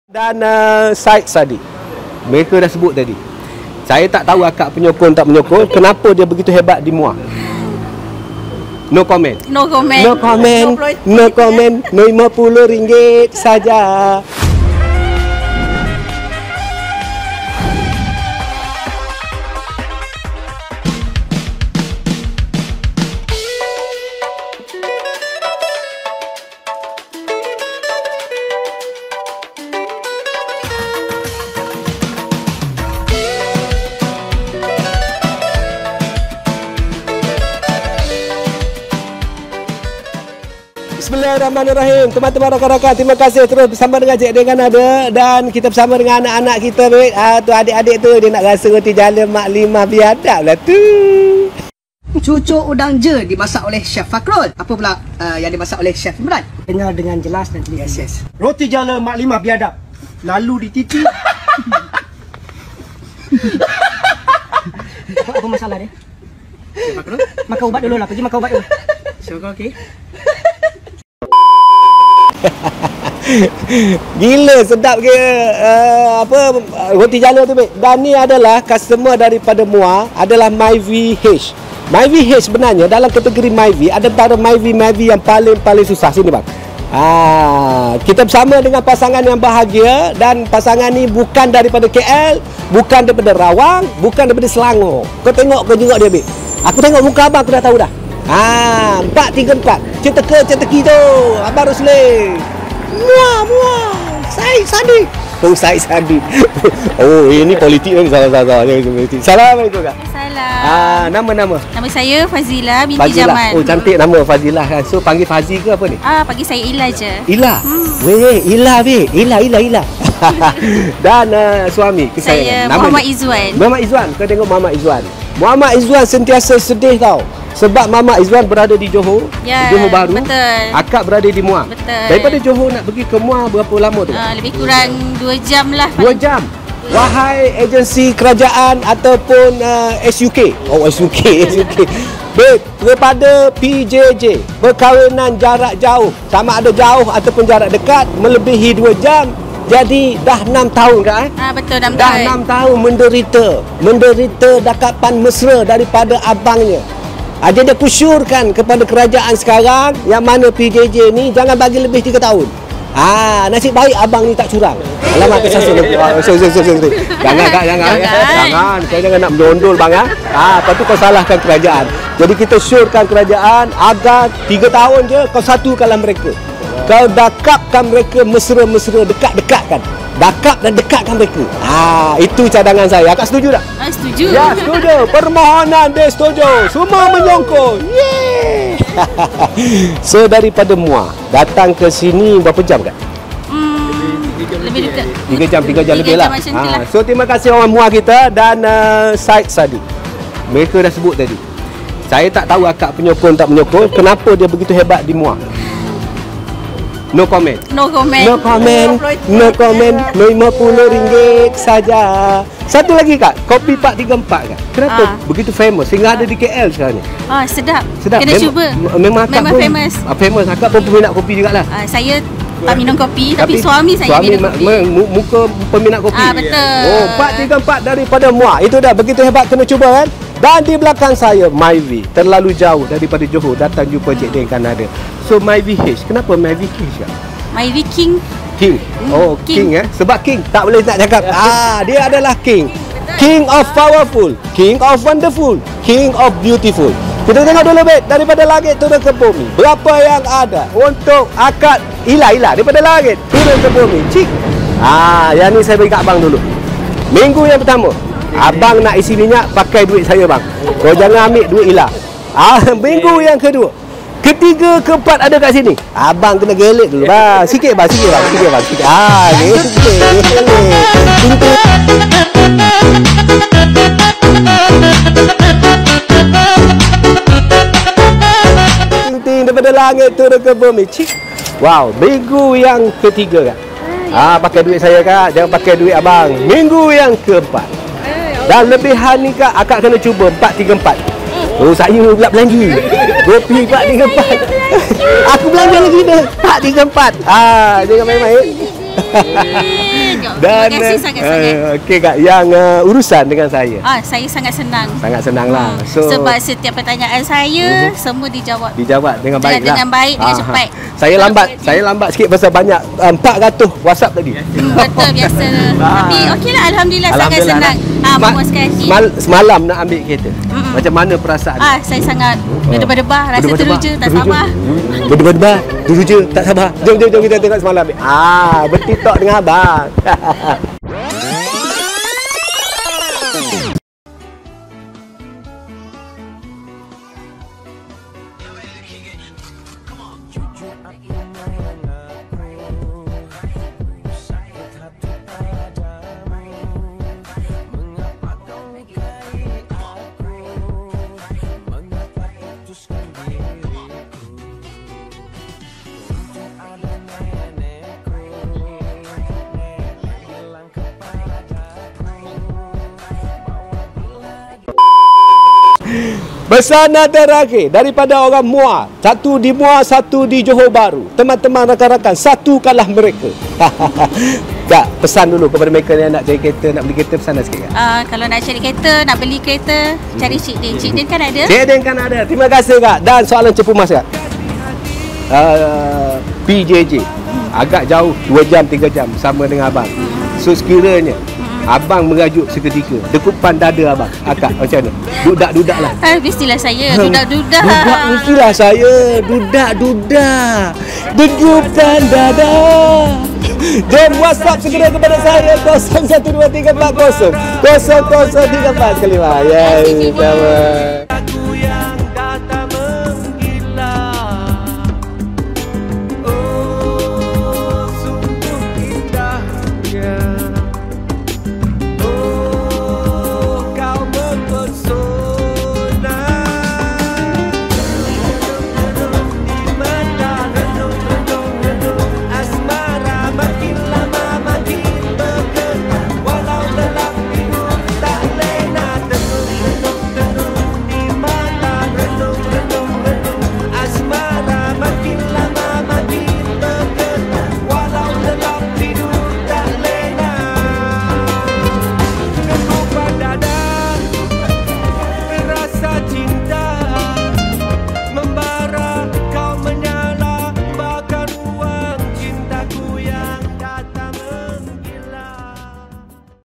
"𝙎𝙖𝙮𝙖 𝙨𝙖𝙣𝙜𝙖𝙩 𝙨𝙚𝙣𝙖𝙣𝙜..𝙨𝙚𝙩𝙞𝙖𝙥 𝙥𝙚𝙧𝙩𝙖𝙣𝙮𝙖𝙖𝙣 𝙨𝙖𝙮𝙖 𝙙𝙞𝙟𝙖𝙬𝙖𝙗 𝙙𝙚𝙣𝙜𝙖𝙣 𝙗𝙖𝙞𝙠 &𝙘𝙚𝙥𝙖𝙩"- 𝙋𝙣.𝙁𝙖𝙙𝙯𝙞𝙡𝙖𝙝,𝙅𝘿𝙏.
Pn.Fadzilah dari Muar,Johor membeli PERODUA MYVI 1.5H BAHARU dengan CHE DIN KAN ADA.

✅IC BOTHSIDE
✅LESEN BOTHSIDE

Sebarang kemusykilan,
sila hubungi